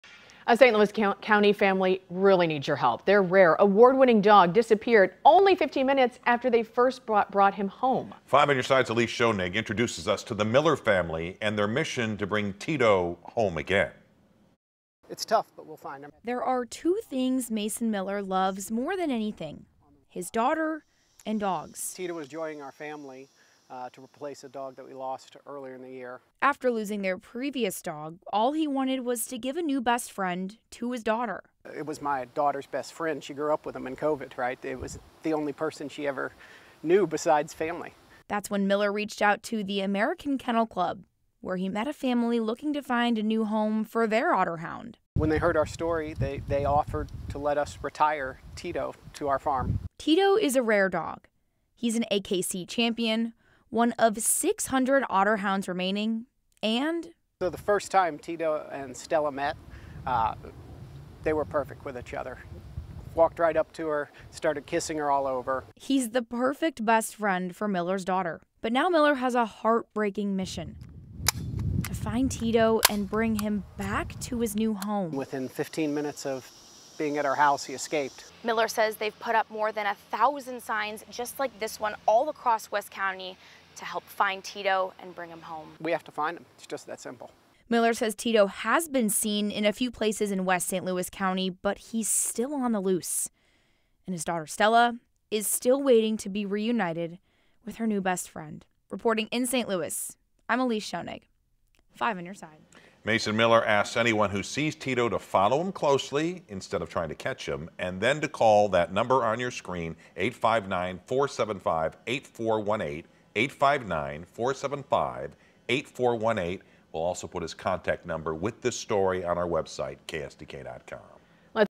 Search underway for rare missing dog in St. Louis area
A search is underway in Missouri for an endangered dog last seen in the St. Louis area missing since Friday.

He was last seen in West County near Laude around 7:30 p.m. on Sunday, the owners said.

5 On Your Side talked to one of the owners, Mason Miller, who said he loves dogs and his daughter more than anything.

"Tito was joining our family to replace a dog we lost earlier in the year," Miller said. "It was my daughter's best friend. She grew up with him in COVID, it was the only other person she knew besides family."